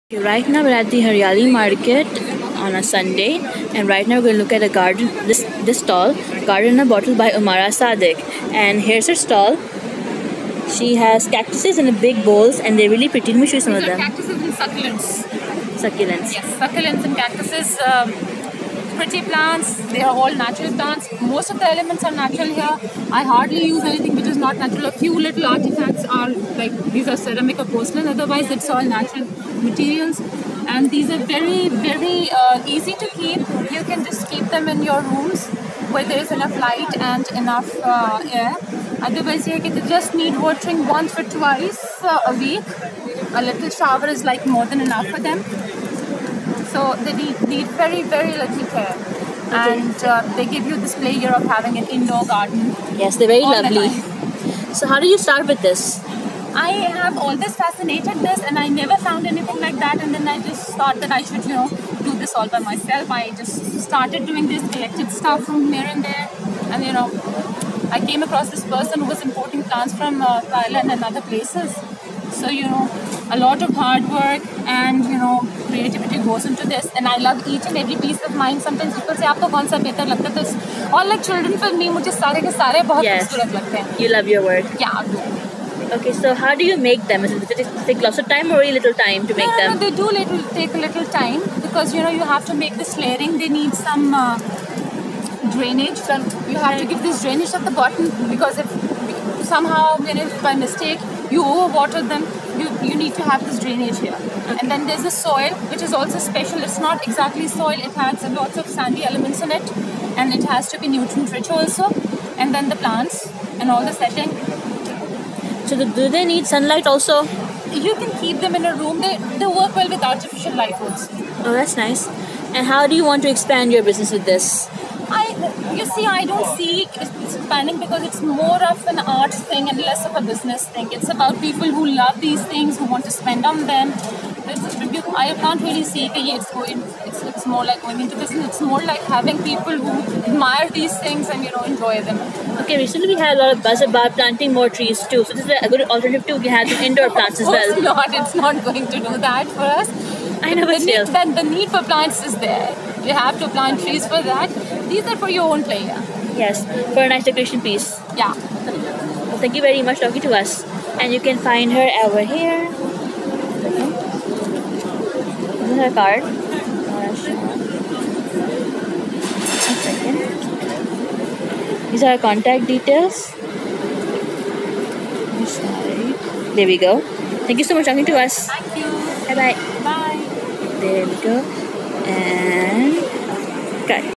Garden in a Bottle by Ummarqh Saqid
This was cute stall with some pretty pots of plants.
And it's nice to see unique things and hobbies that people do.
Very quirky.